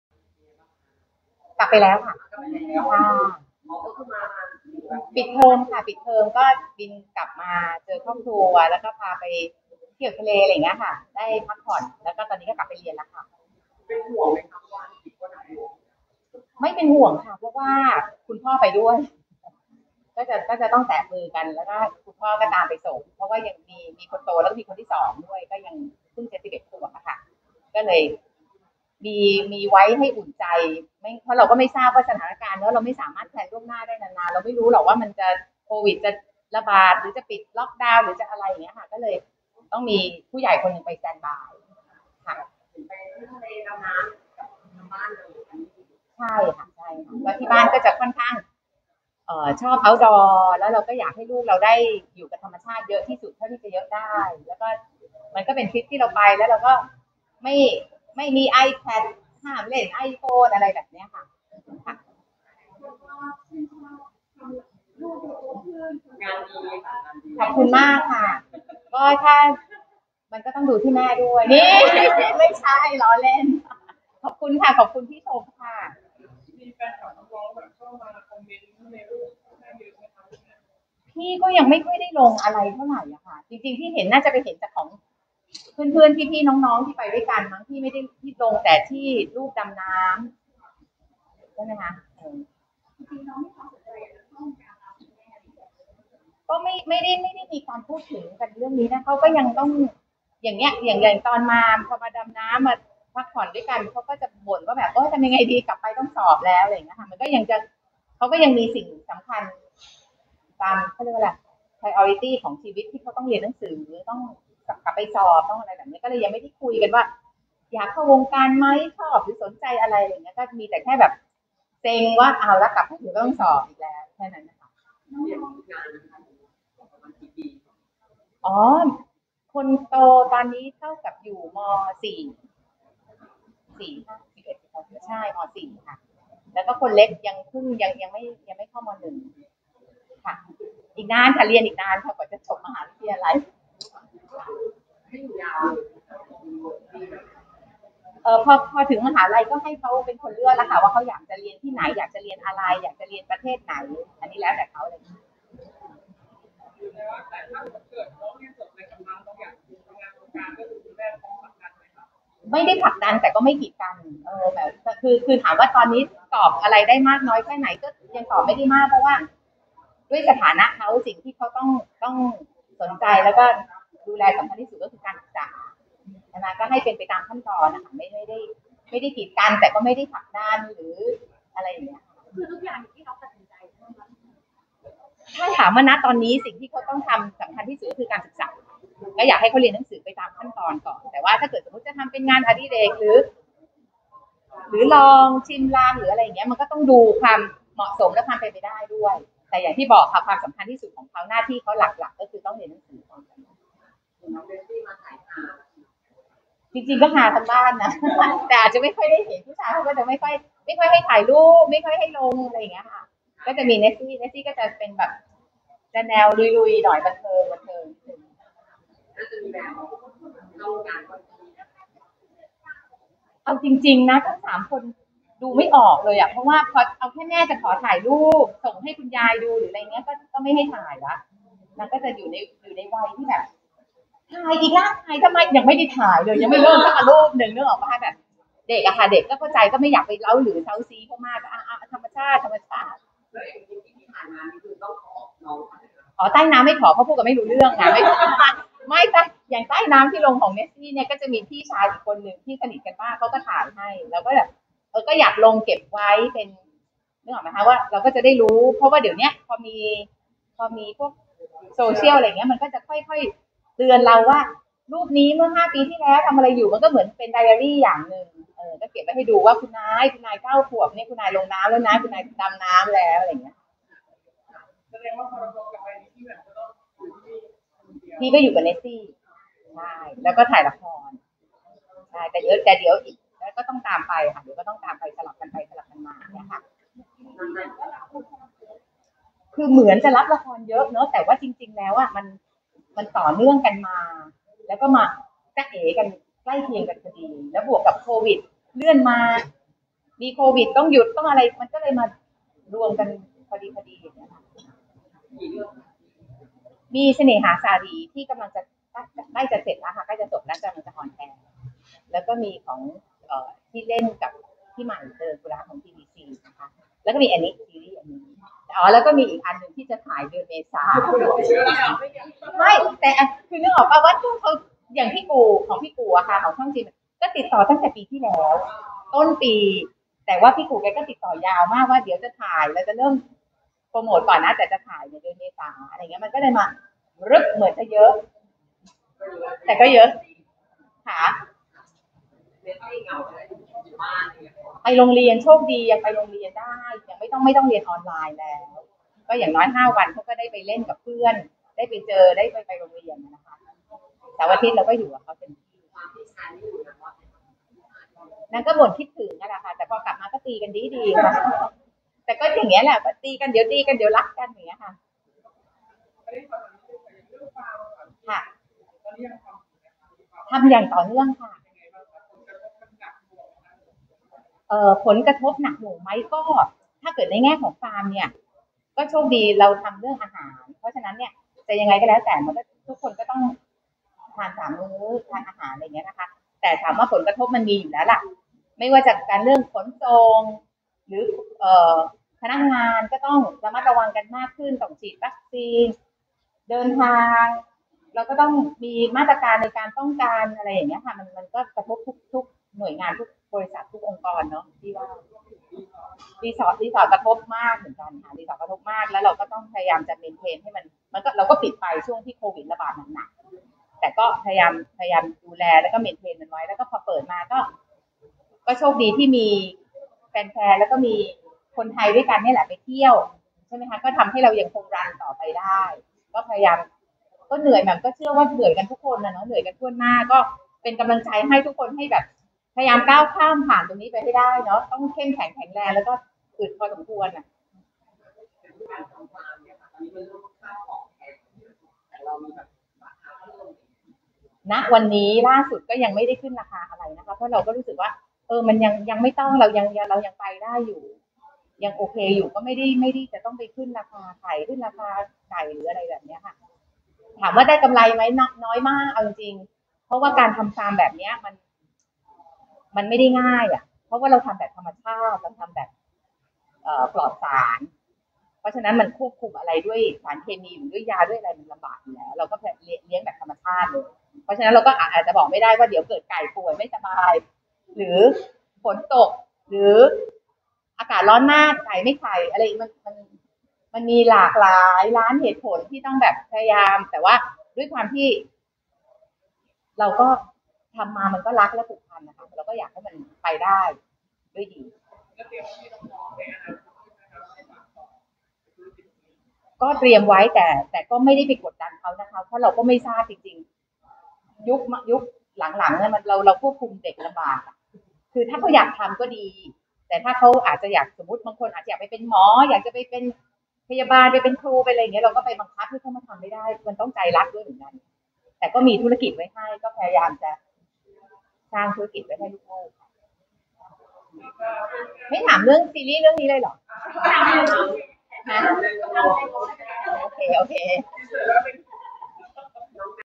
“แหม่ม” คัทลียา แมคอินทอช รับละคร 4 เรื่องแบบรัวๆ ส่วนน้องแมคให้เขาเน้นการเรียนก่อน
งานบวงสรวงซีรีส์ "เส้นลองจิจูดที่180องศาลากผ่านเรา"
วันที่ 16 มกราคม พ.ศ.2565 ณ บริเวณ ศาลพระพรหมหน้าตึก ทิปโก้ พระราม6